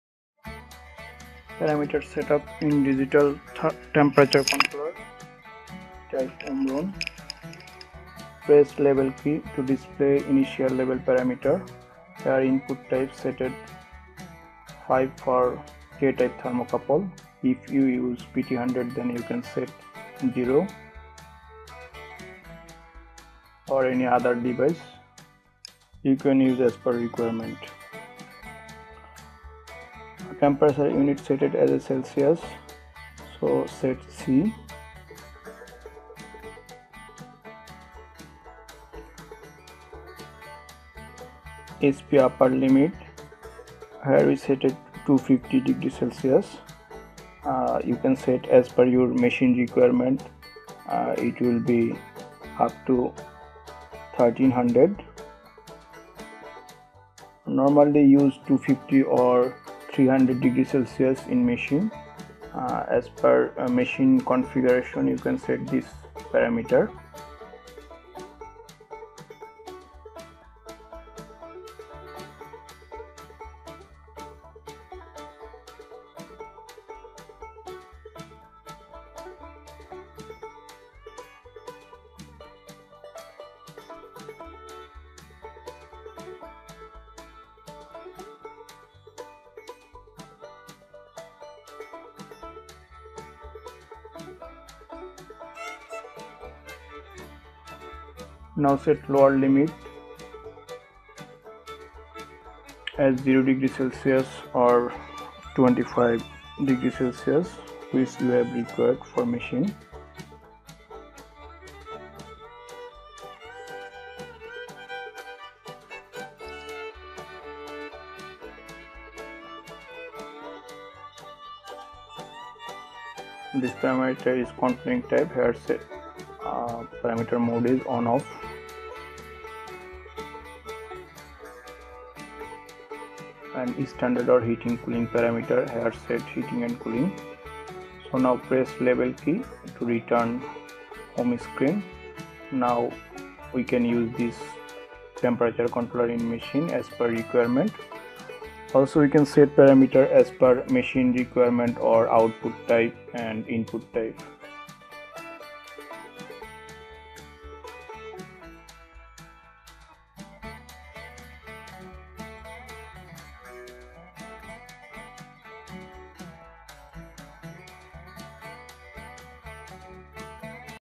𝐏𝐚𝐫𝐚𝐦𝐞𝐭𝐞𝐫 𝐬𝐞𝐭𝐮𝐩 𝐨𝐟 𝐎𝐦𝐫𝐨𝐧 𝐃𝐢𝐠𝐢𝐭𝐚𝐥 𝐓𝐞𝐦𝐩𝐞𝐫𝐚𝐭𝐮𝐫𝐞 𝐂𝐨𝐧𝐭𝐫𝐨𝐥𝐥𝐞𝐫 𝐄𝟓𝐀𝐙 𝐄𝟓𝐄𝐙 𝐄𝟓𝐂𝐙
To move to the initial setting level from the operation level or the adjustment level, press the O key for at least 3 seconds. The PV display
flashes after 1 second. This level is for specifying the input type and
selecting the control method, control period, setting direct/reverse action,
and setting the alarm types. You can move to the advanced function setting level or communications setting level from this level. To return to the
operation level, press the O key for at least 1 second. To move to the
communications setting level, press the O key for less than 1 second.
(When moving from the initial setting level to the operation level, all the
indicators will light.)

Note:  Pressing the O key for at least 3 seconds in the operation level's
auto/manual switching display will move to the manual control level, and not the initial setting level.

electrical learning
electronic learning
electric circuits,
beginner electronics,